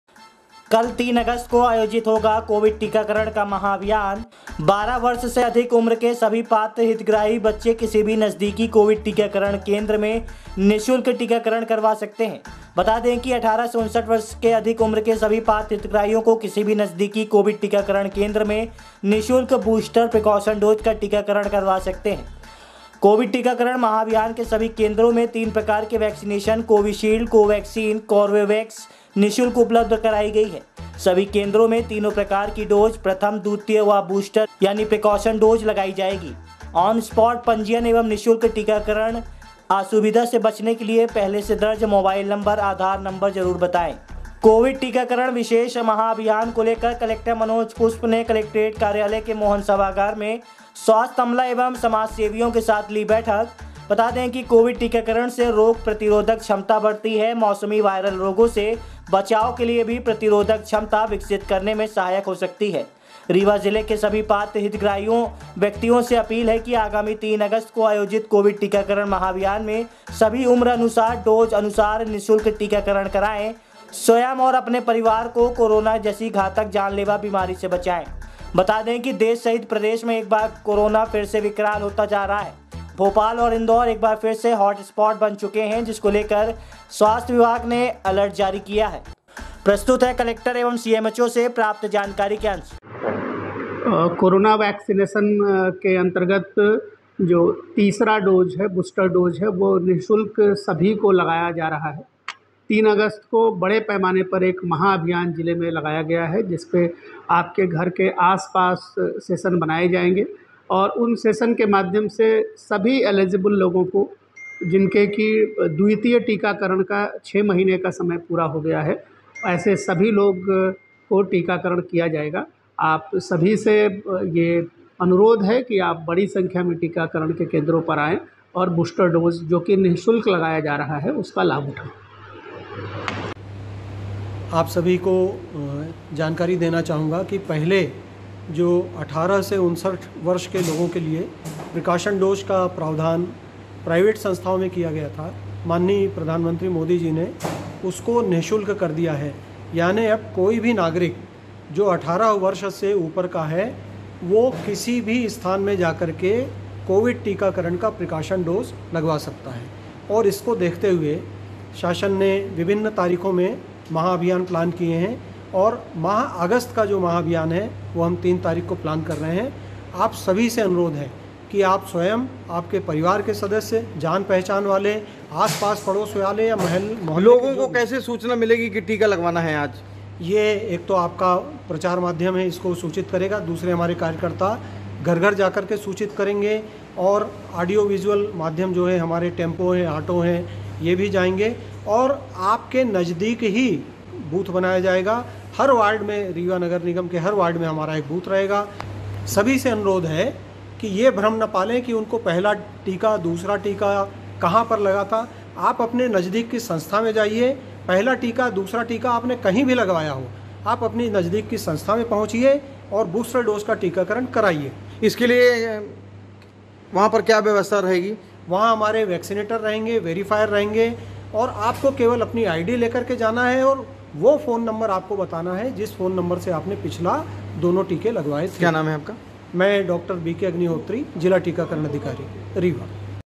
अमृत महोत्सव अंतर्गत कोरोना से बचाव हेतु कल आयोजित होगा कोविड-19 टीकाकरण का महाभियान,,, कलेक्टर अपील
*अमृत महोत्सव अंतर्गत कोरोना से बचाव हेतु कल आयोजित होगा कोविड-19 टीकाकरण का महाभियान,,, कलेक्टर ने की आम जनता से यह अपील,,,,,,देखें सिटी न्यूज़ वैन रीवा की ये खबर।*